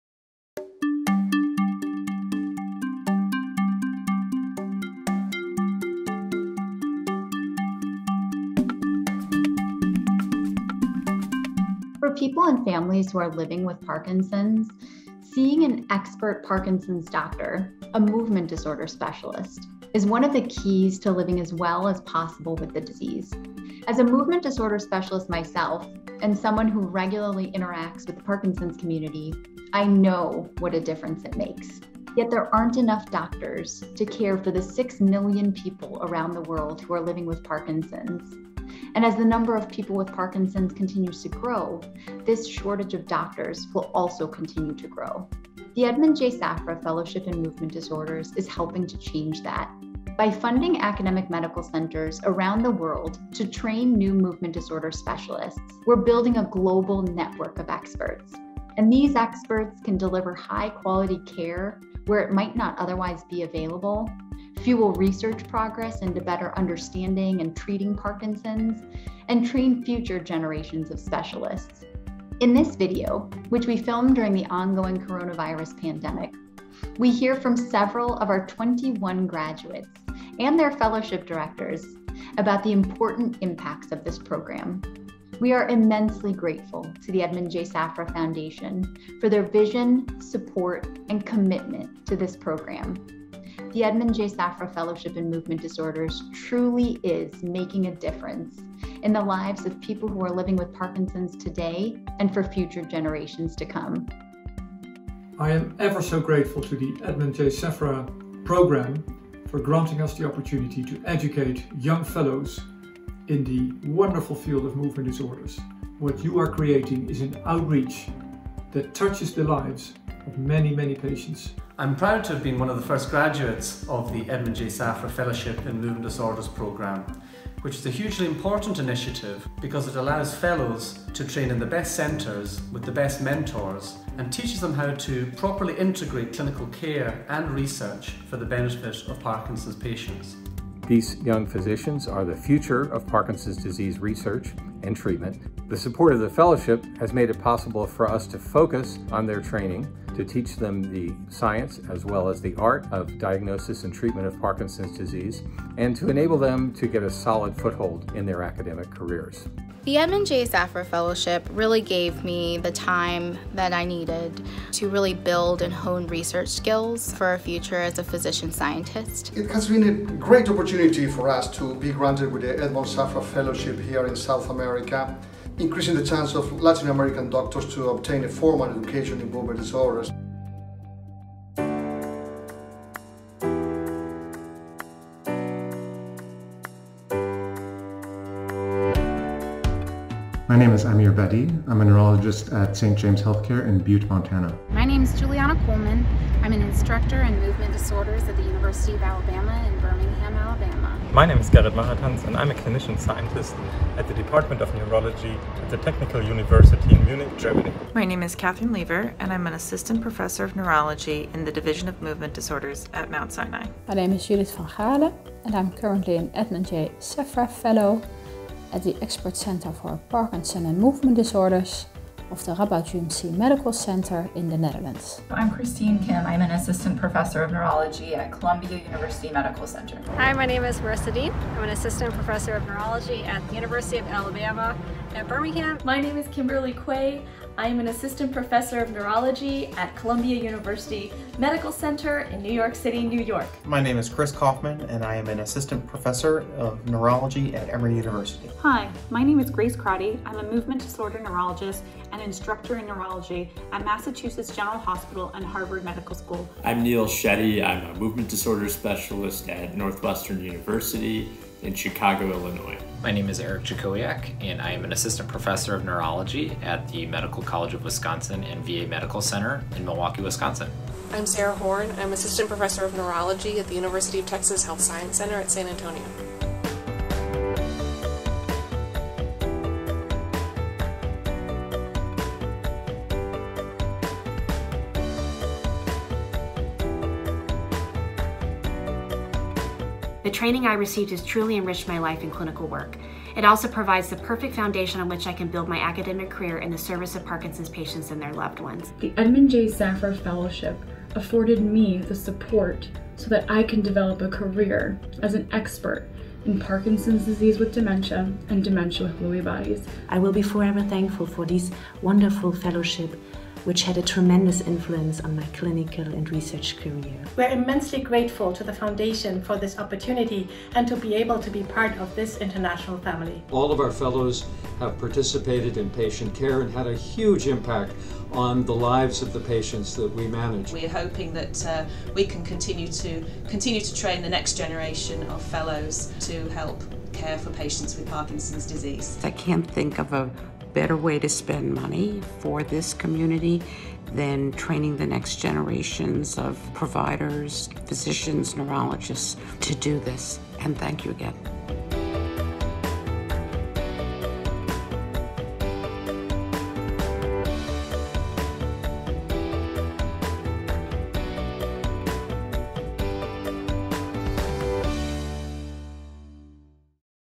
The Edmond J. Safra Fellowship in Movement Disorders: Improving Lives through Research and Care
The Michael J. Fox Foundation, with partner the Edmond J. Safra Foundation, is building a global network of movement disorder specialists through The Edmond J. Safra Fellowship in Movement Disorders. In this video, hear from several of our 21 alumni and their mentors about the program’s impact.

Learn more about The Edmond J. Safra Fellowship in Movement Disorders